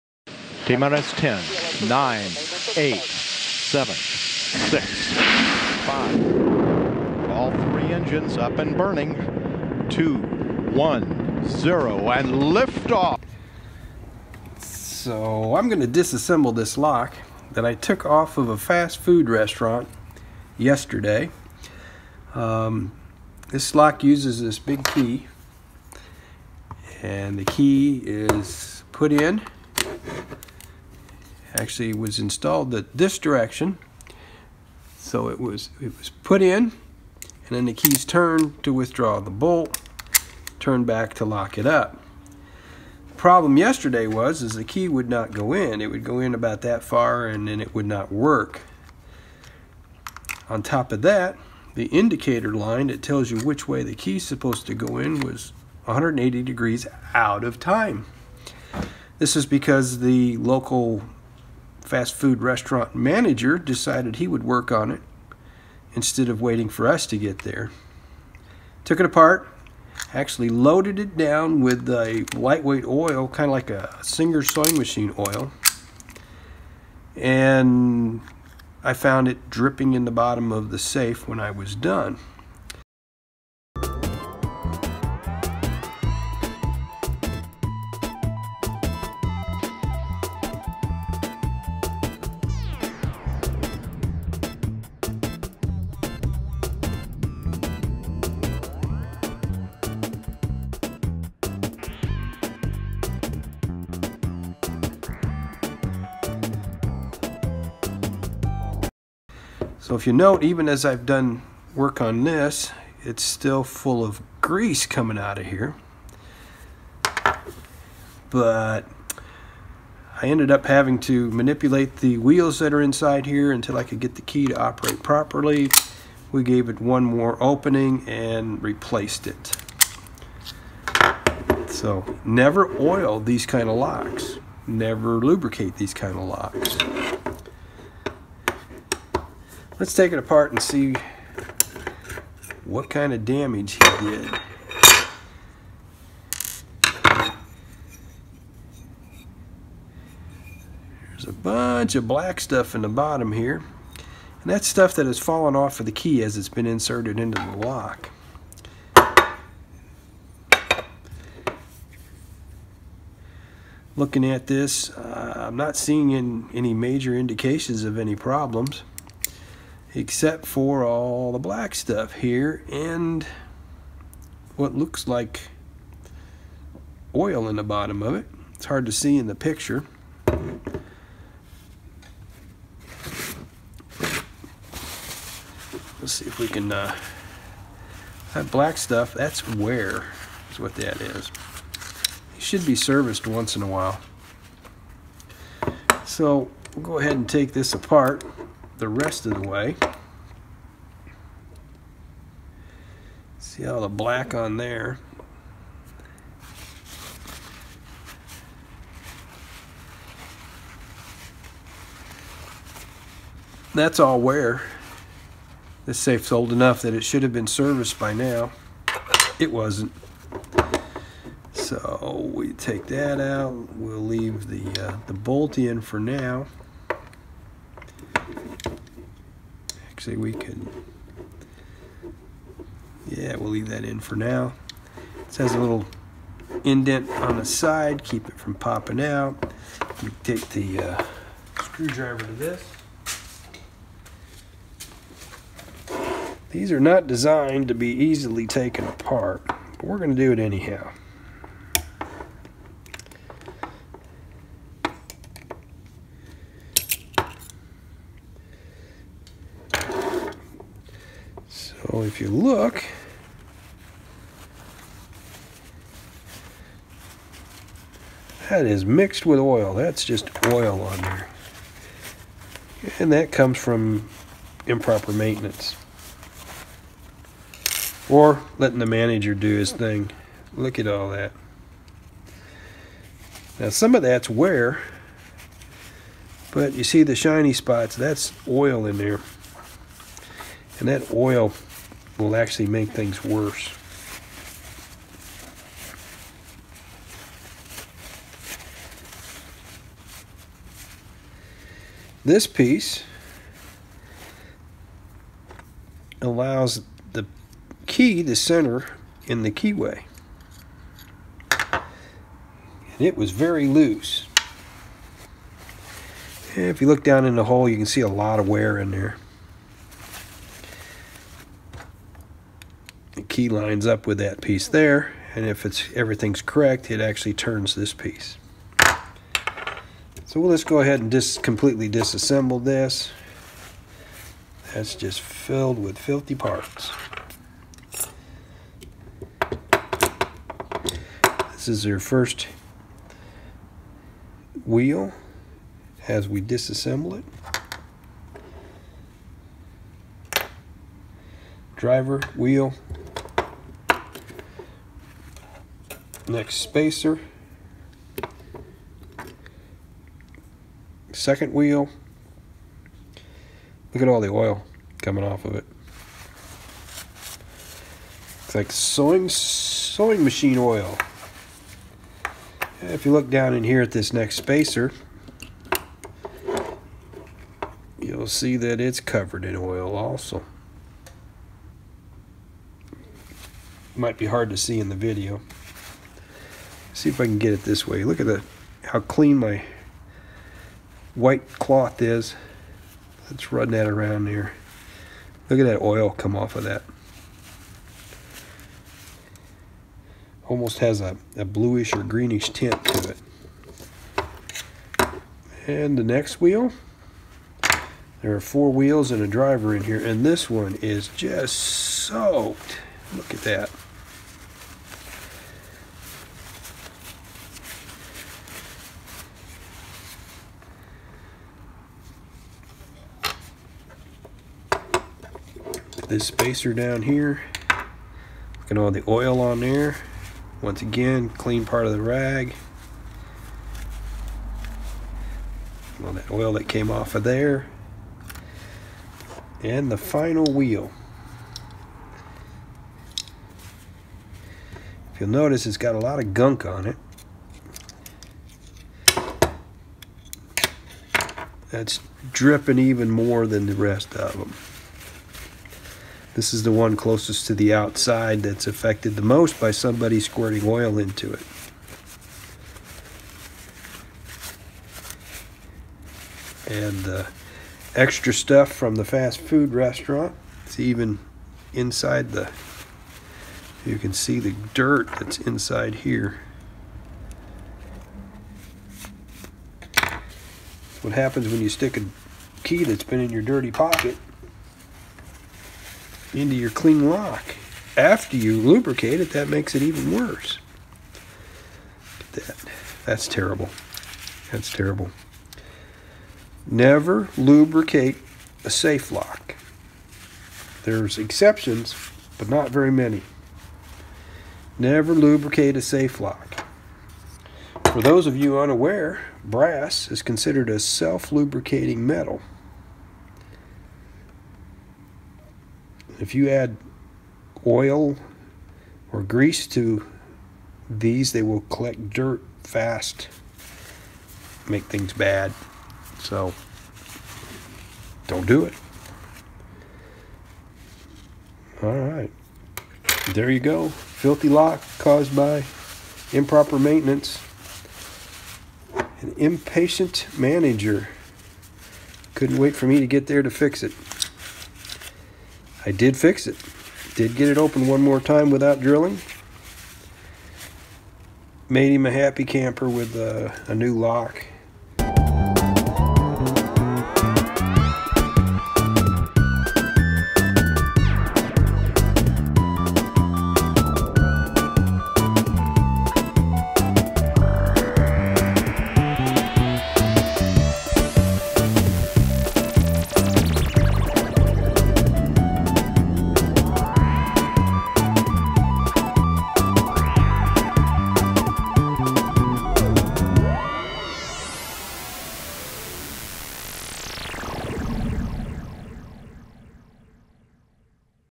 An Oily, Dirty Little Lock
This LaGard (model 2270) key operated safe lock was removed from a fast food restaurant safe after it failed to allow the key to enter the keyway.  There were a few surprises left behind by the store manager that might have made this safe opening worse than it actually was.  Oil, dirt, cooking flour, improper maintenance, and an overzealous restaurant manager did not make for a good combination (pun intended) for this safe lock.

LaGard 2270 key-operated lock with 3" keys.